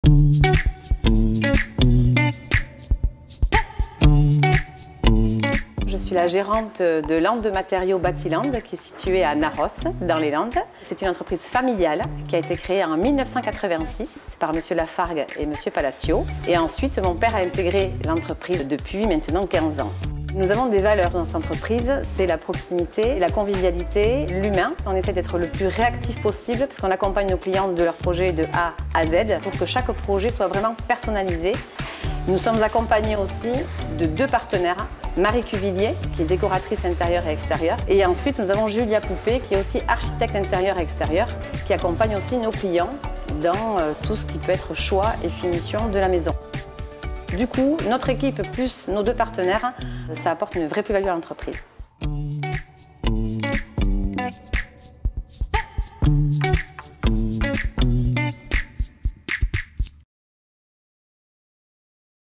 Landes matériaux - Magasin matériaux de construction : Narrosse
Dans le marché dacquois du négoce de matériaux de construction depuis trente ans, notre entreprise a toujours œuvré pour rester en phase avec les valeurs qui ont inspiré sa naissance.

Compétence, professionnalisme, service et accompagnement personnalisés avec en plus la convivialité d’une équipe proche de vous et à votre écoute.

Chez nous le client grand ou petit est toujours écouté, considéré et tout sera mis en œuvre pour répondre à sa demande, dans les délais les plus brefs.

Notre adhésion au groupement Batiland et à la centrale CMEM nous permettent de vous proposer une gamme de produits très large, issue de fournisseurs experts et leaders du marché, avec toutes les garanties de qualité que vous êtes en droit d’attendre et d’exiger. Tout en restant, bien-sûr, parmi les meilleurs rapports qualité-prix du marché.

Notre service de fourni-posé pourra vous faire bénéficier d’importantes aides fiscales et de réductions de TVA.

Alors une maison individuelle en projet, un agrandissement ou rénovation de votre logement, une terrasse à aménager, un garage ou abri de jardin en vue, une menuiserie ou volet roulant à changer, un faitage ou une couverture à refaire, un projet d’isolation par l’extérieur, une salle de bains à remettre au goût du jour, un assainissement à refaire, une clôture à concevoir, un portail à changer ou tout simplement un conseil technique à demander, n’hésitez pas et poussez la porte de notre magasin, nous essaierons de satisfaire votre demande avec tous nos moyens et bien-sûr avec le sourire.